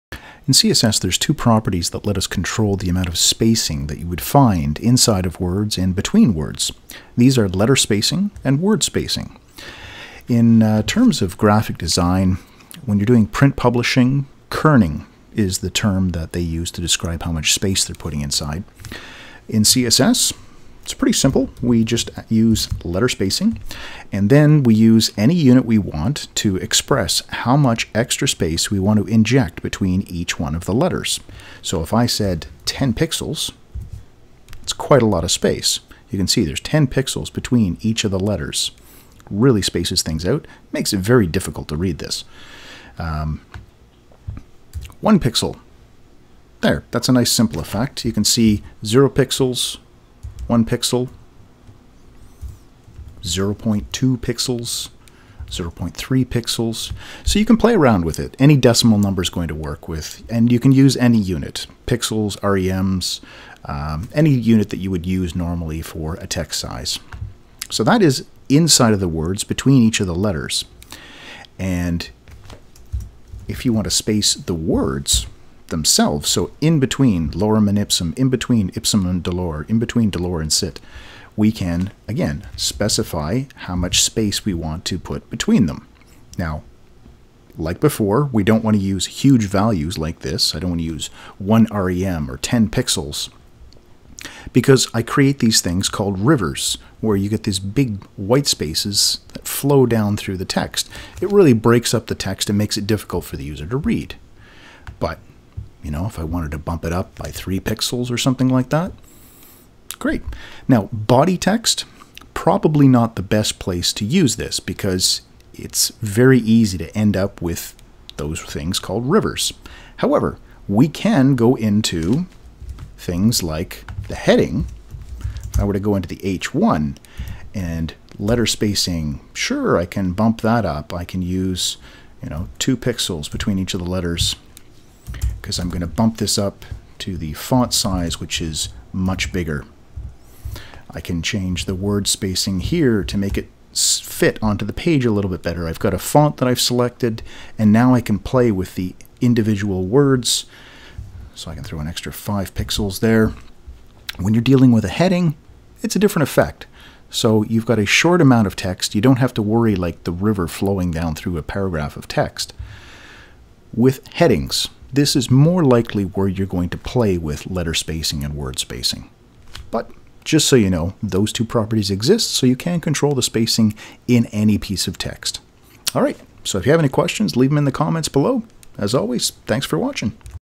CSS Letter and Word Spacing
There are two CSS properties that let us control the kerning for text on  our webpages. Ideally you would be using these inside your headings to make them fill a space better.

the properties covered in this tutorial are `letter-spacing` and `word-spacing`.